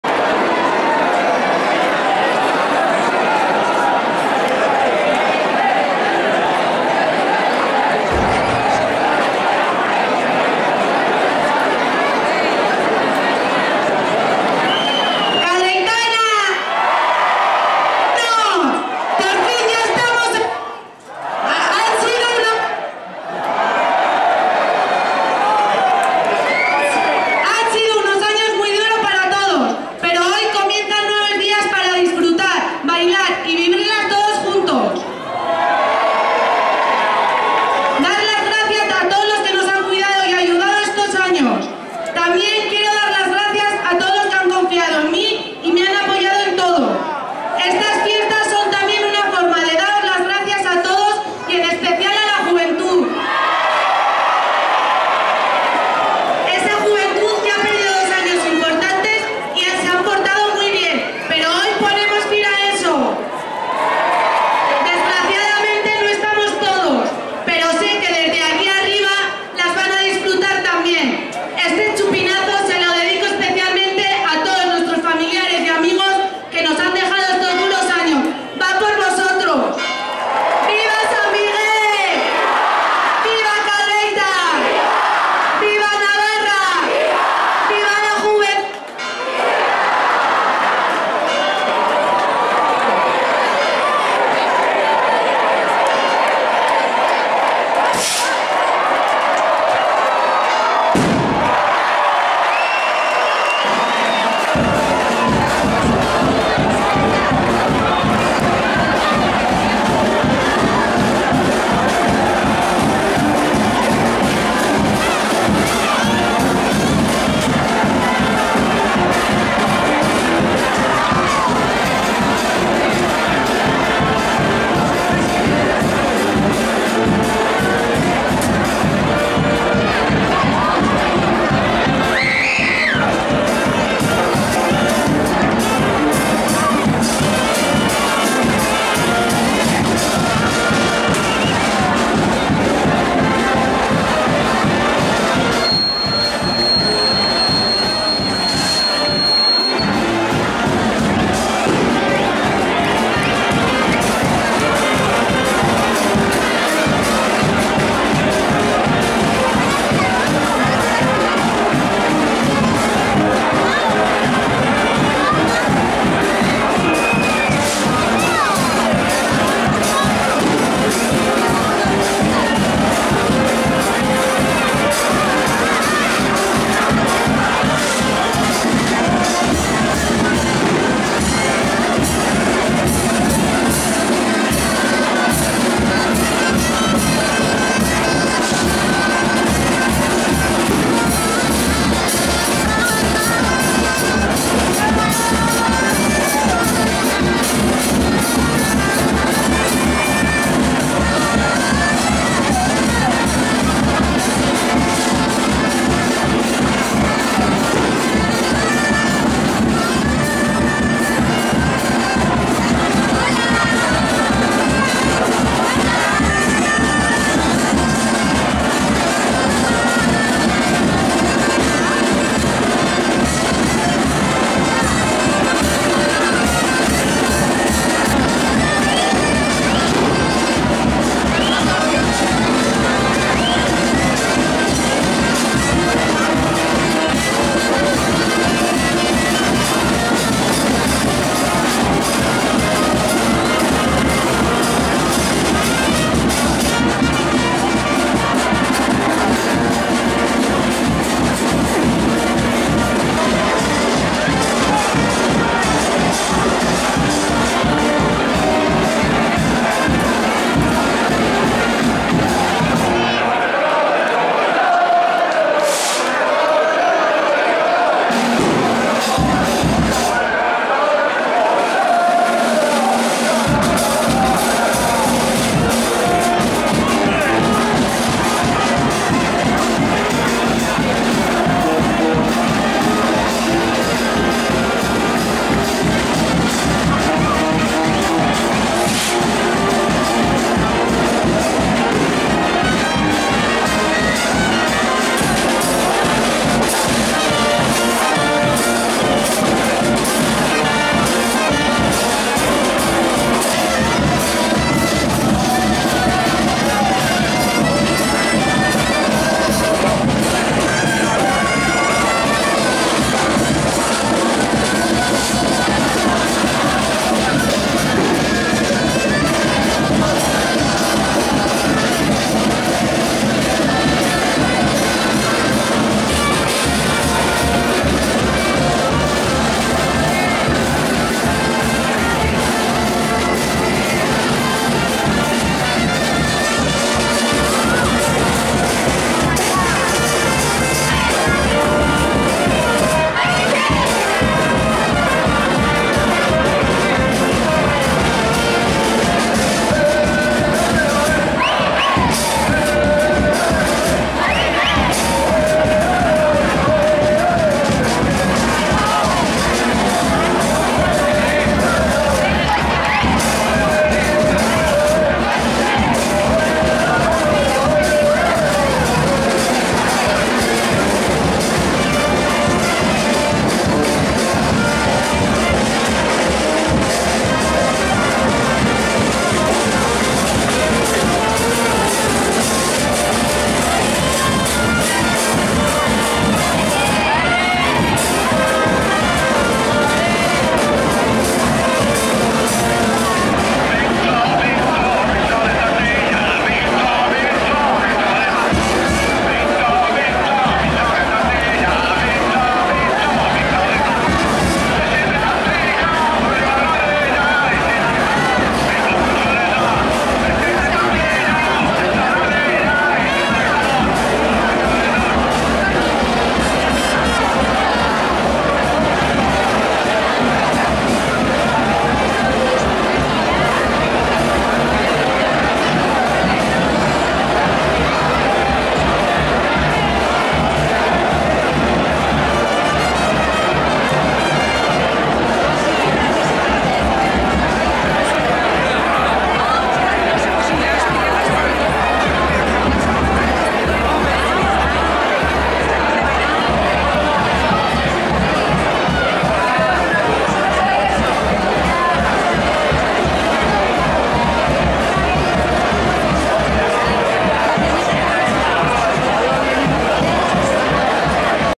Chupinazo de Cadreita - 2022
Imágenes del chupinazo de inicio de las fiestas de Cadreita del año 2022.